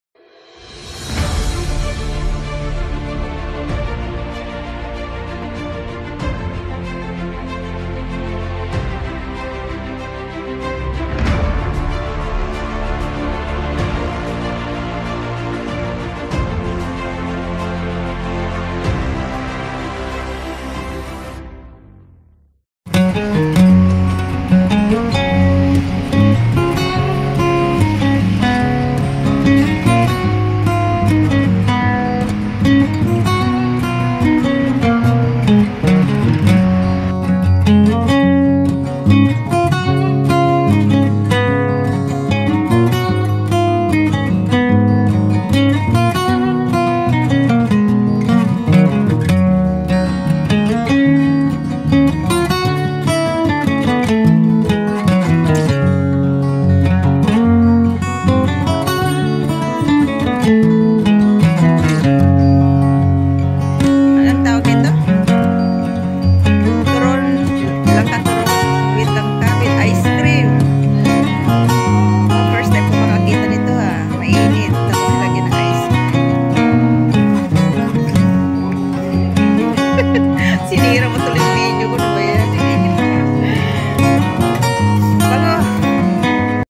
TURON LANGKA with ICE CREAM
Bonding time with my unica hija Sheena Pearl Perez.